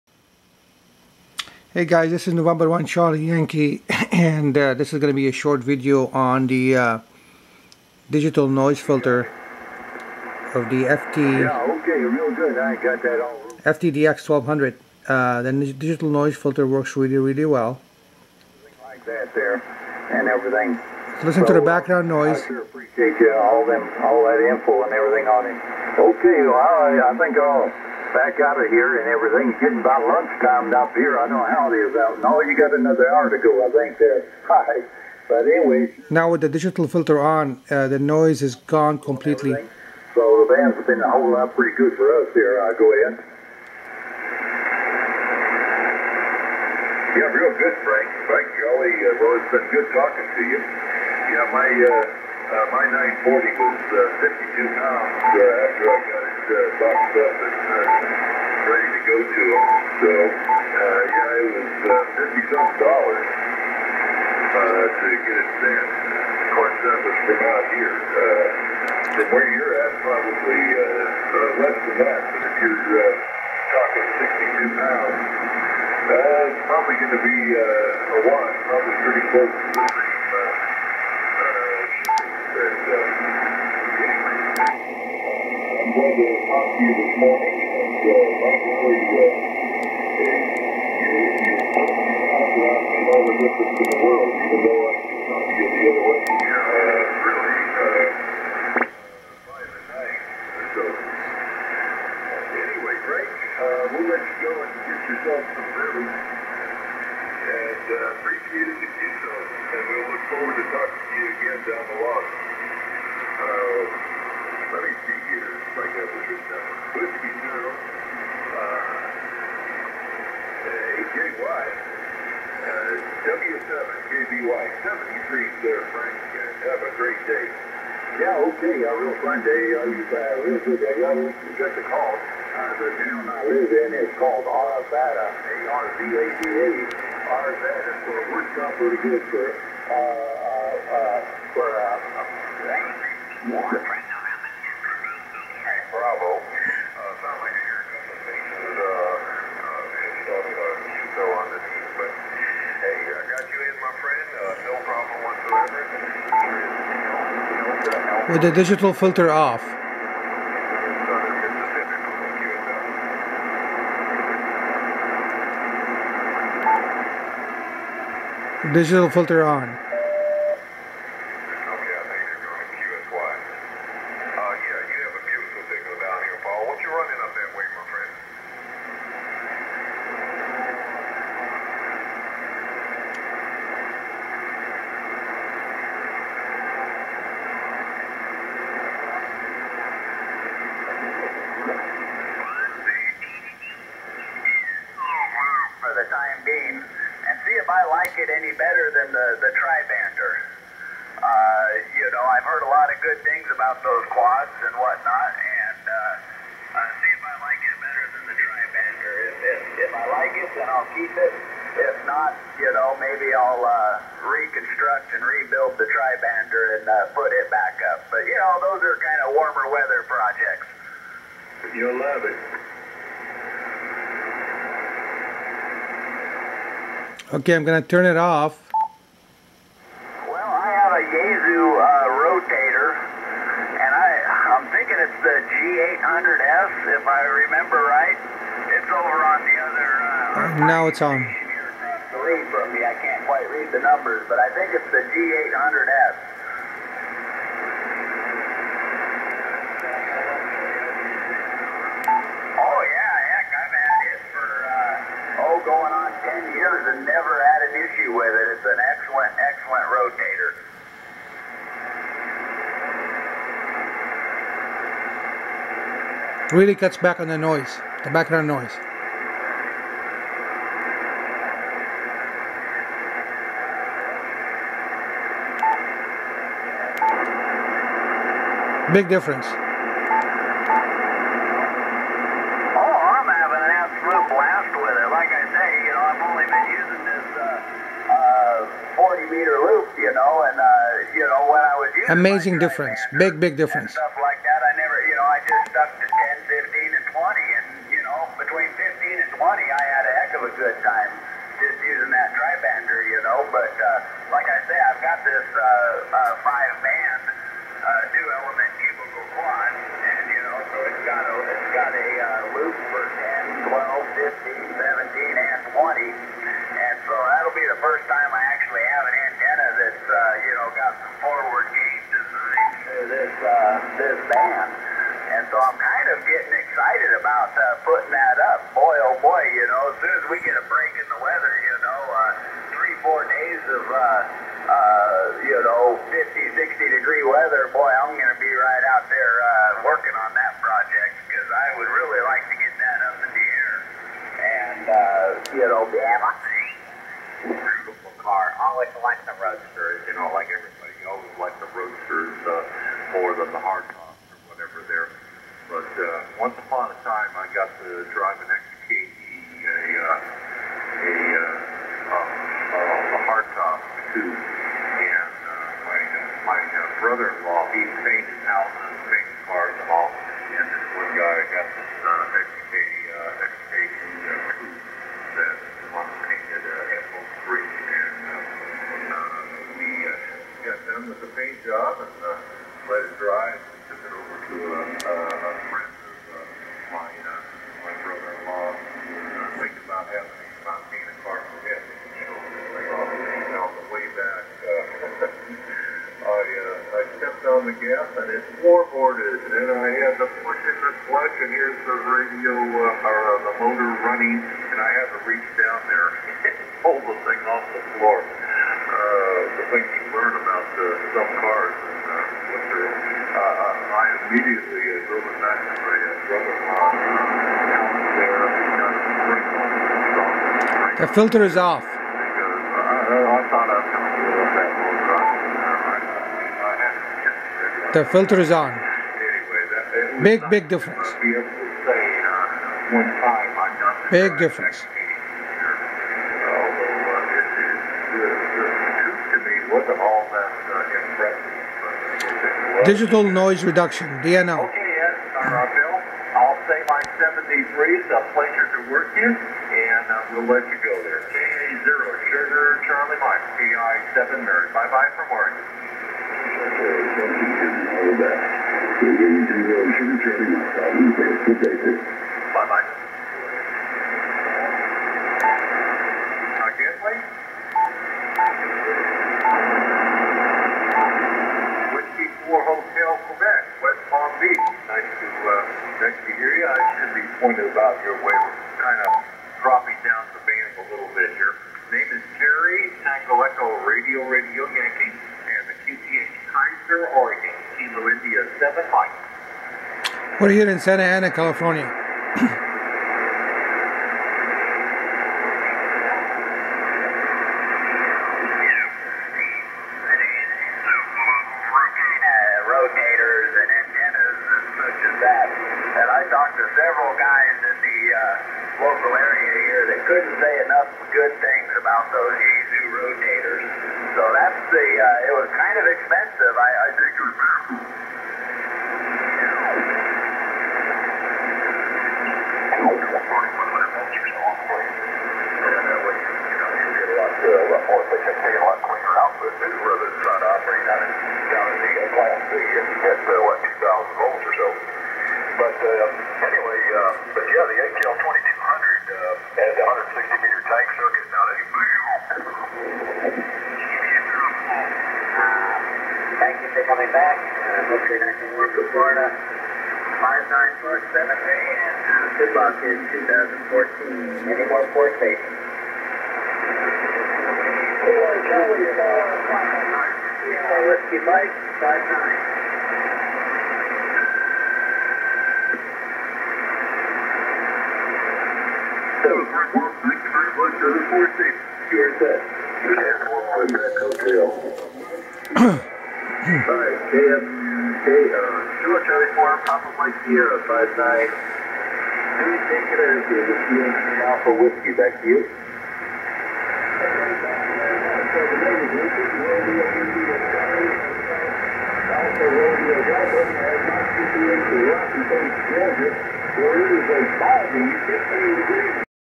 Yaesu FTDX 1200 DNR Digital Noise Reduction
A snapshot of the DNR filter at work with the Yaesu FTDX 1200 HF/6 meters all mode transceiver. The DNR filters operate at the IF stage and are very efficient in reducing background noise without sacrificing the fidelity of the received signal.